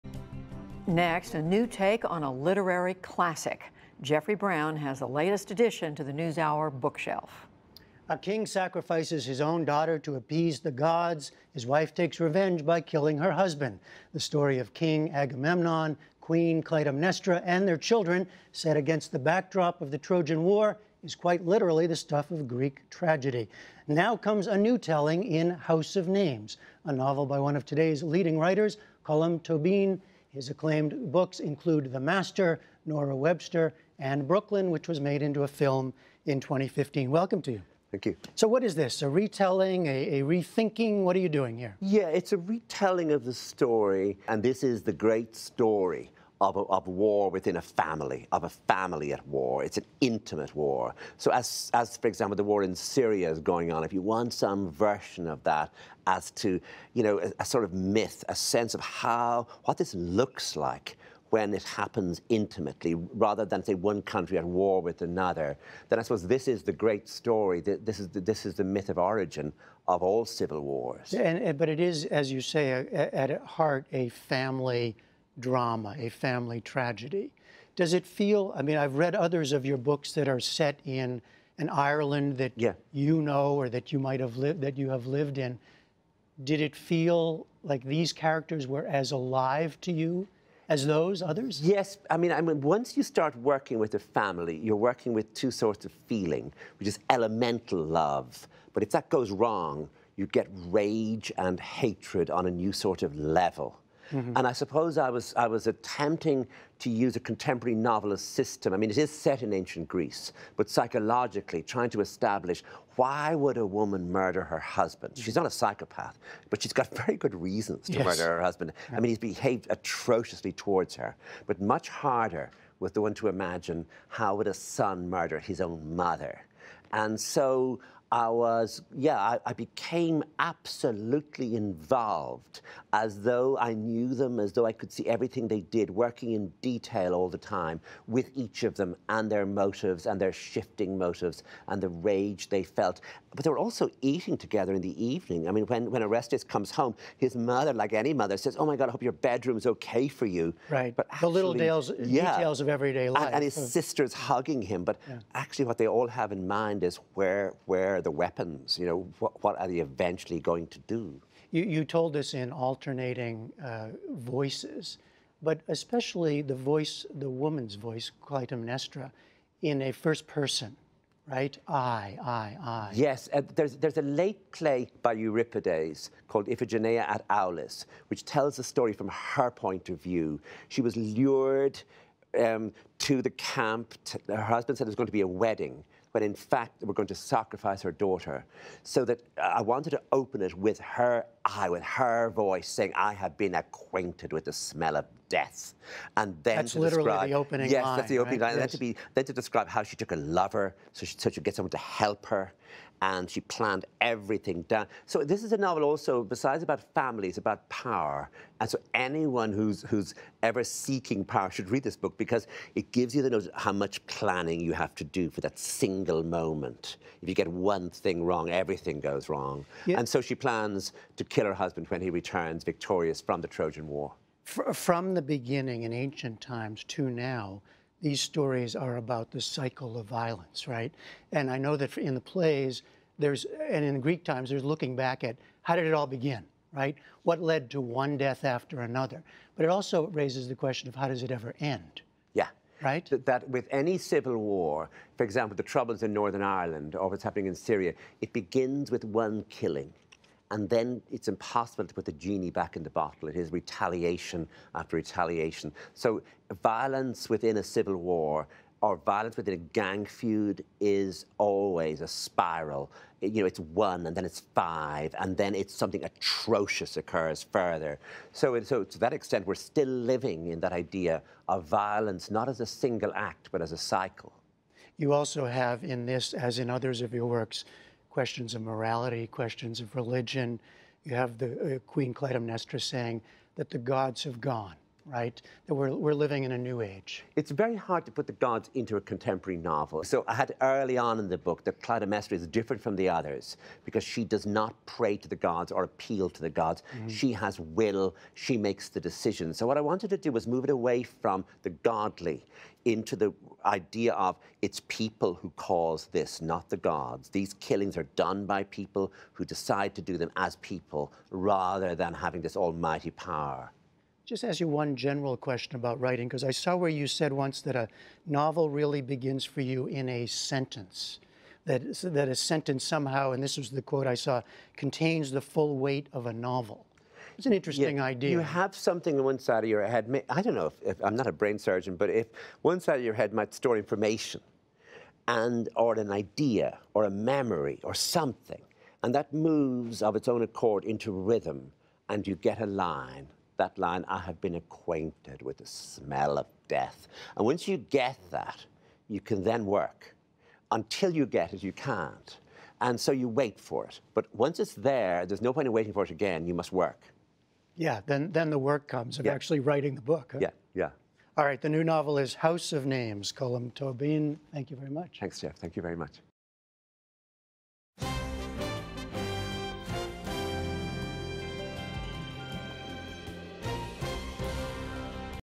Colm Toibin sees the 'origin of all civil wars' in this Greek tragedy
In the new novel "House of Names," one of today's leading contemporary writers looks back to the Trojan War and Greek mythology for inspiration. Colm Toibin joins Jeffrey Brown to discuss why he wanted to write a novel about a family drama in ancient Greece and the connection to civil wars in our own time.